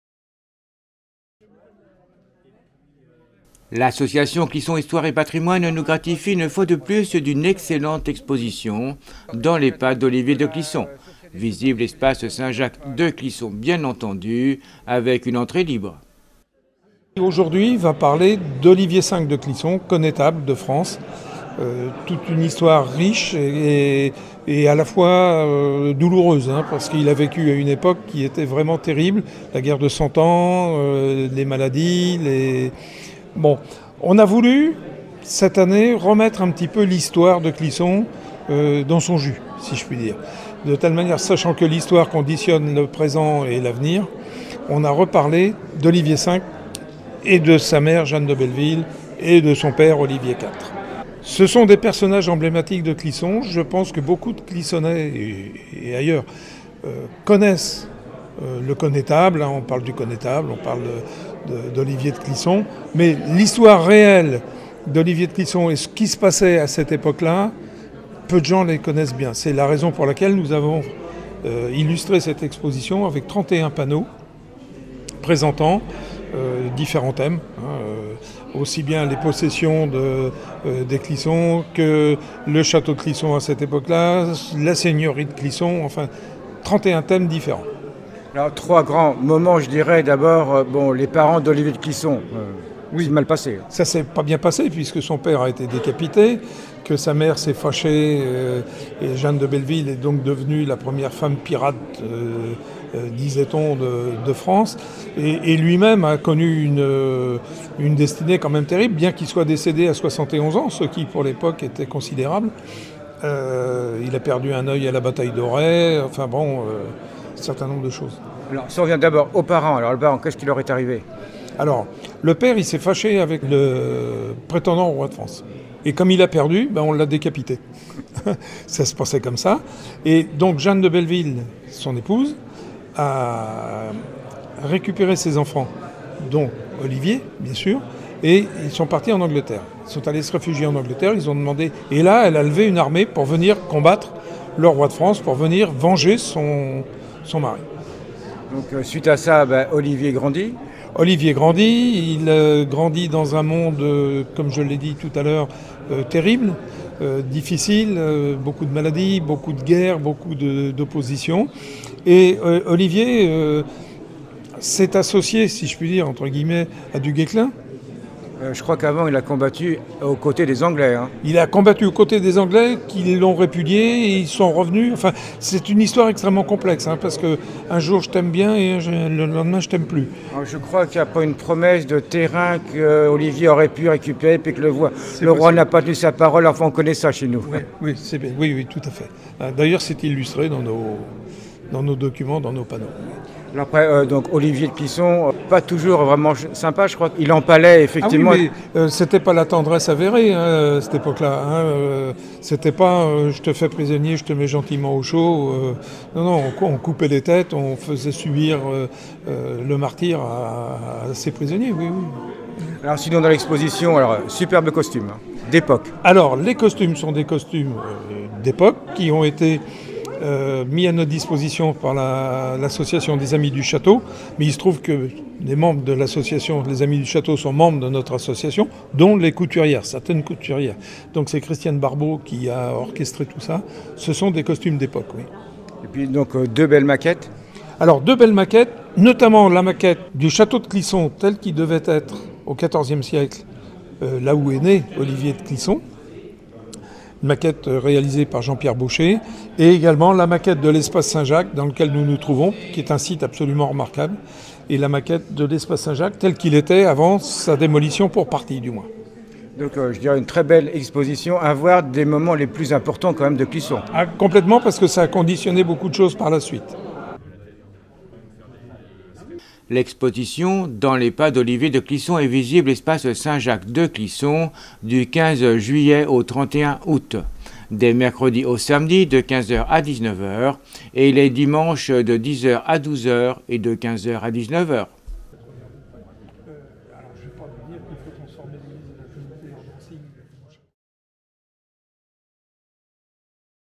Dans les Pas d’Olivier de Clisson : Clisson Histoire et Patrimoine
Dans les Pas d’Olivier de Clisson
L’association Clisson Histoire et Patrimoine nous gratifie une fois de plus d’une excellente exposition « Dans les Pas d’Olivier de Clisson » visible Espace Saint Jacques de Clisson bien entendu avec une entrée libre.